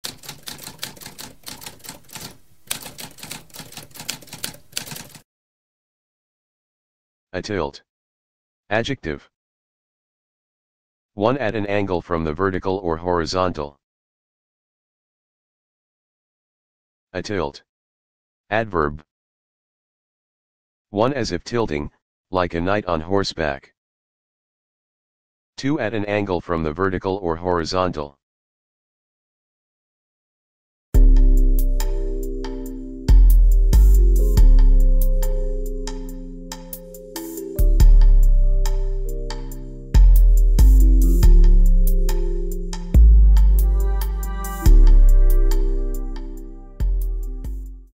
What does atilt mean?
A spoken definition of atilt.

Intro Sound:
Typewriter - Tamskp